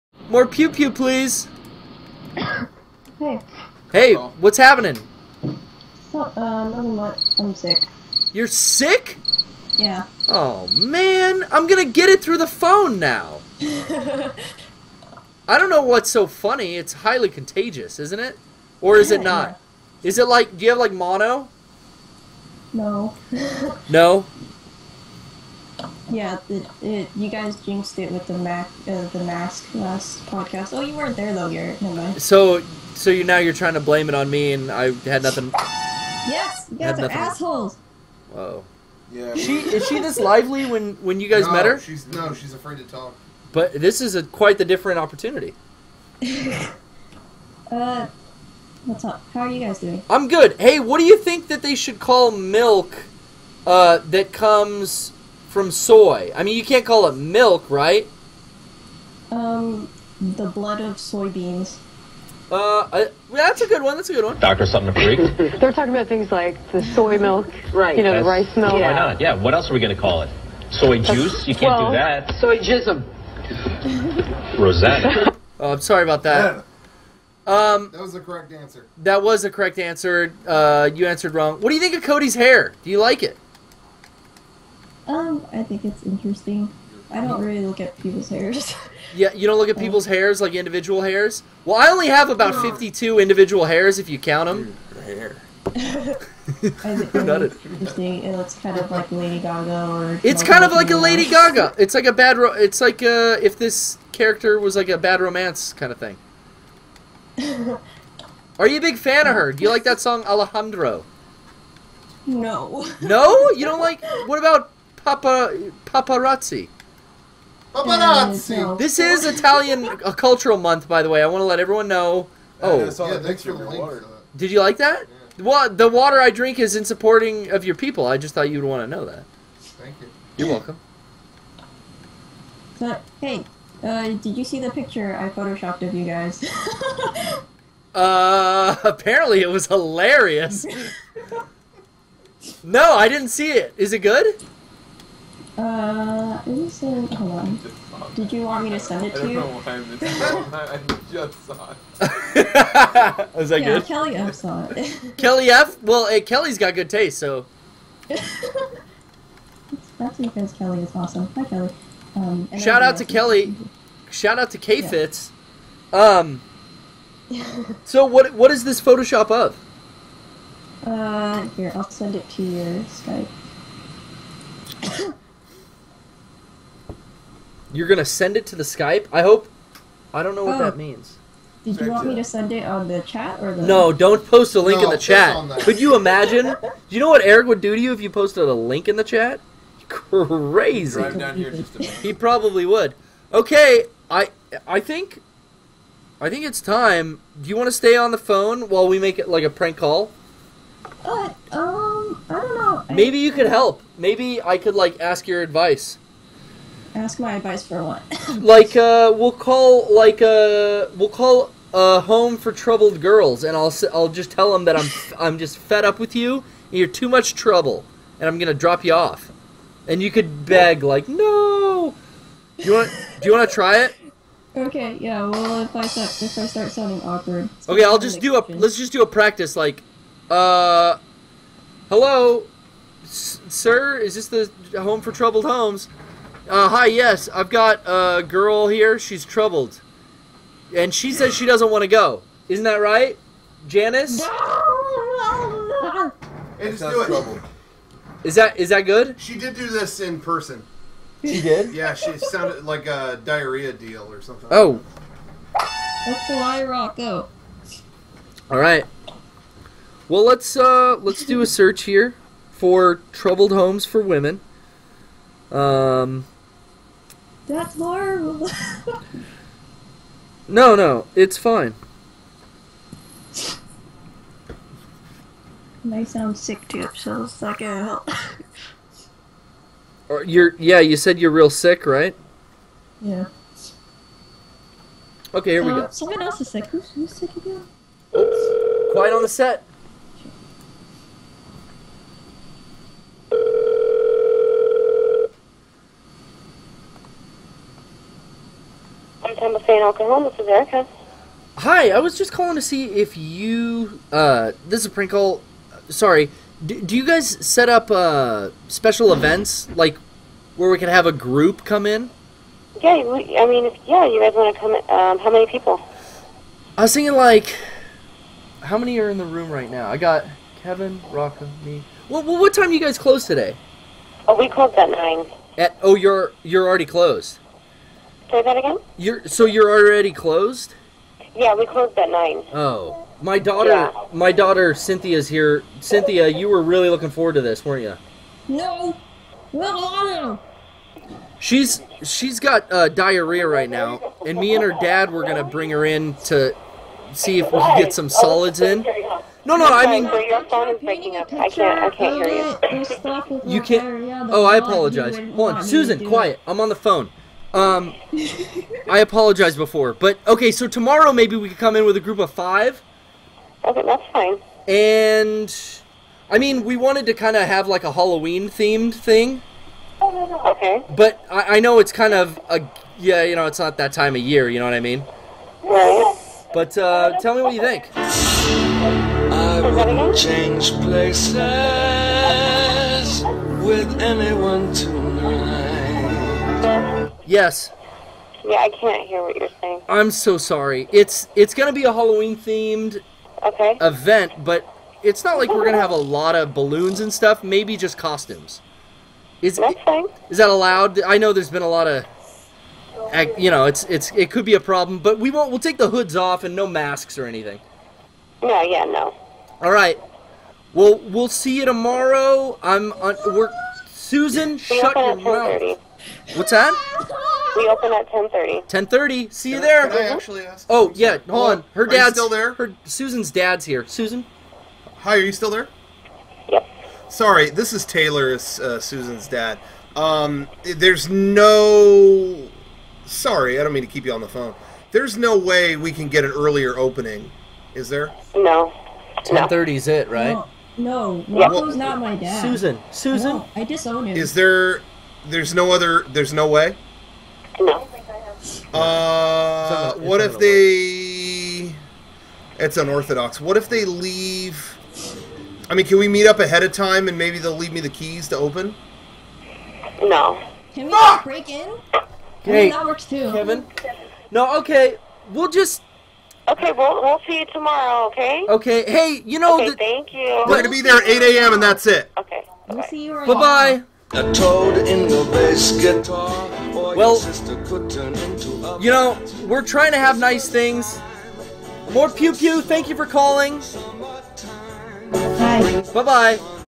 Mega64 Poorly Played Stream 43 - Prank Call 10 with PewPew
This is the second time on the Poorly Played Stream in which a caller has contributed to the Prank Call segment.